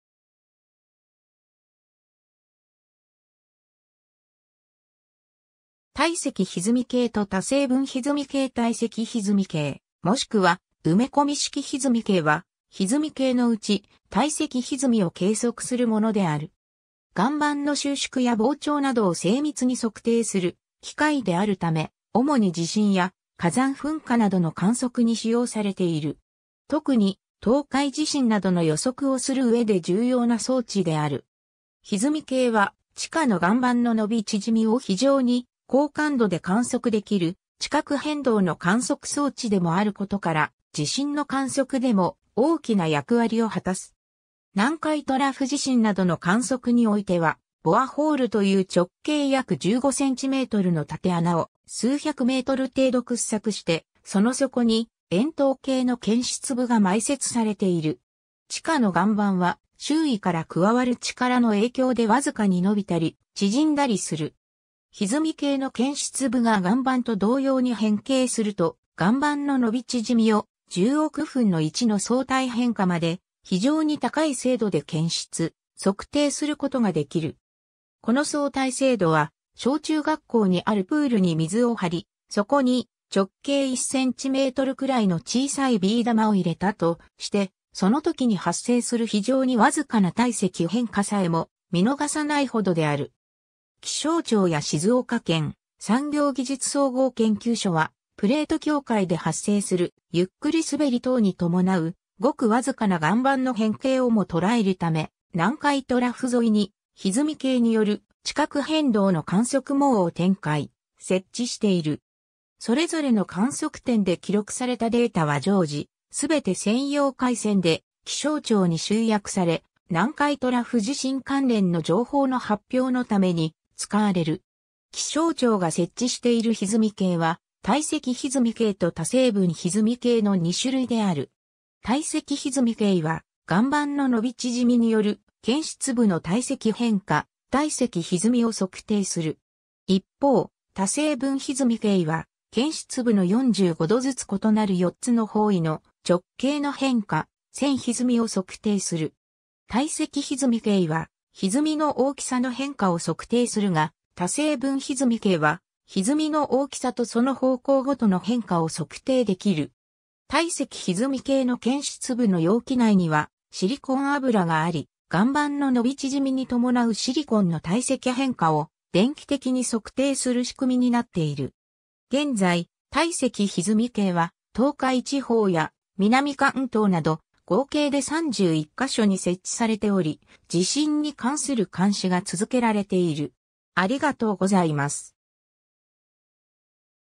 体積ひずみ計
体積ひずみ計と多成分ひずみ計 体積ひずみ計（たいせきひずみけい）もしくは埋込式ひずみ計は、ひずみ計のうち、体積ひずみを計測するものである。
岩盤の収縮や膨張などを精密に測定する器械であるため、主に地震や火山噴火などの観測に使用されている。
特に東海地震などの予測をする上で重要な装置である。
 ひずみ計は、地下の岩盤の伸び・縮みを非常に高感度で観測できる地殻変動の観測装置でもあることから、地震の観測でも大きな役割を果たす。
南海トラフ地震などの観測においては、ボアホールという直径約15cmの縦穴を数100メートル程度掘削して、その底に円筒形の検出部が埋設されている。
地下の岩盤は、周囲から加わる力の影響で僅かに伸びたり縮んだりする。
ひずみ計の検出部が岩盤と同様に変形すると、岩盤の伸び縮みを、10億分の1の相対変化まで非常に高い精度で検出・測定することができる。
この相対精度は、小中学校にあるプール（長さ25m、幅10m、深さ1.5m程度）に水を張り、そこに直径1cmくらいの小さいビー玉を入れたとして、その時に発生する非常に僅かな体積変化さえも見逃さないほどである。
 気象庁や静岡県、産業技術総合研究所は、プレート境界で発生するゆっくりすべり等に伴うごく僅かな岩盤の変形をも捉えるため、南海トラフ沿いに、ひずみ計による地殻変動の観測網を展開・設置している。
それぞれの観測点で記録されたデータは常時、全て専用回線で気象庁に集約され、南海トラフ地震関連の情報の発表のために使われる。
 気象庁が設置しているひずみ計は、体積ひずみ計と多成分ひずみ計の2種類である。
体積ひずみ計は、岩盤の伸び縮みによる検出部の体積変化「体積ひずみ」を測定する。
一方、多成分ひずみ計は、検出部の45度ずつ異なる4つの方位の直径の変化「線ひずみ」を測定する。
体積ひずみ計はひずみの大きさの変化を測定するが、多成分ひずみ計はひずみの大きさとその方向ごとの変化を測定できる。
体積ひずみ計の検出部の容器内にはシリコン油があり、岩盤の伸び縮みに伴うシリコンの体積変化を電気的に測定する仕組みになっている。
現在、体積ひずみ計は東海地方や南関東など、合計で31箇所に設置されており、地震に関する監視が続けられている。